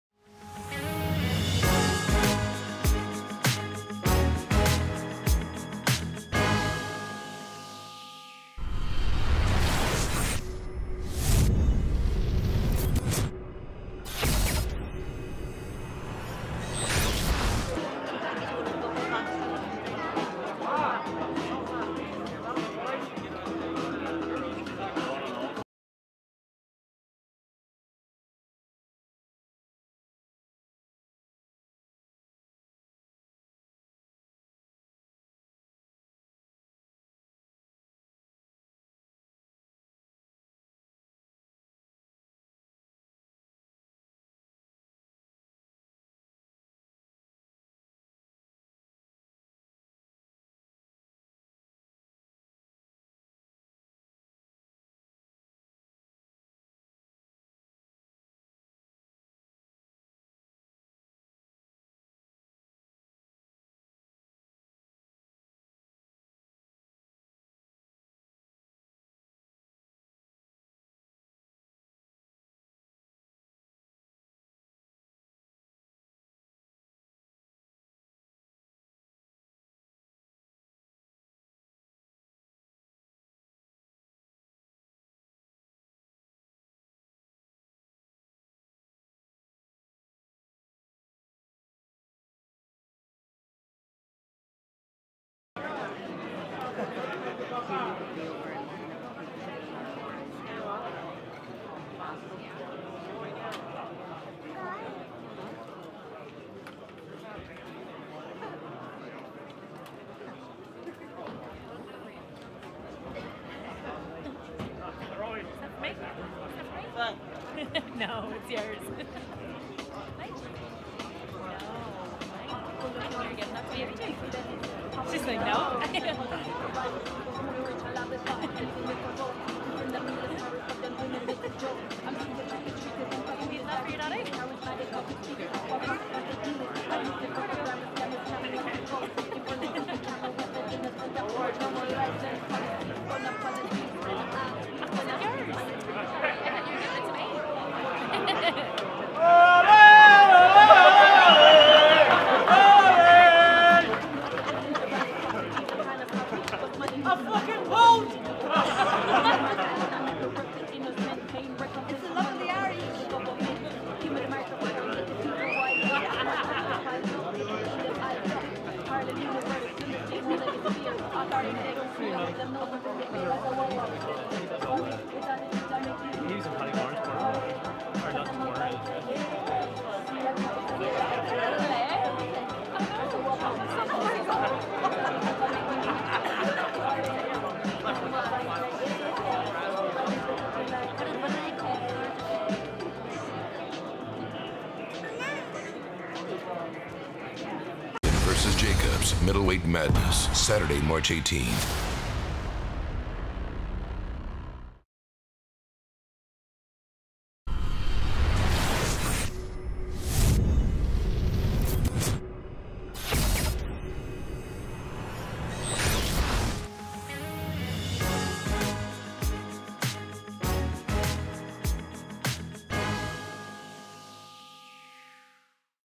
'IT'S THE CHAMP!' - LEGENDARY LEPRECHAUN 'CLIFFY' LOVING LIFE WITH THE TOP RANK GIRLS / CONLAN DEBUT
'IT'S THE CHAMP!' - LEGENDARY LEPRECHAUN 'CLIFFY' LOVING LIFE WITH THE TOP RANK GIRLS / MICHAEL CONLAN DEBUT